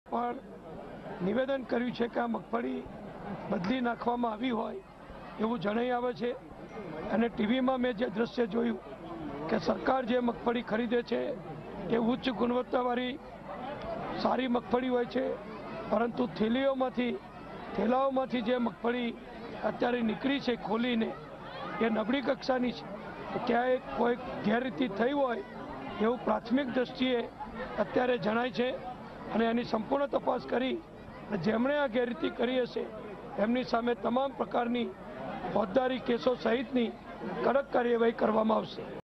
Junagadh મગફળી કૌભાંડ મુદ્દે DyCM Nitin Patel એ કહ્યું, કે જવાબદારો સામે કડક પગલાં લેવાશે | VTV
Junagadh મગફળી કૌભાંડ મુદ્દે DyCM Nitin Patel એ કહ્યું, કે જવાબદારો સામે કડક પગલાં લેવાશે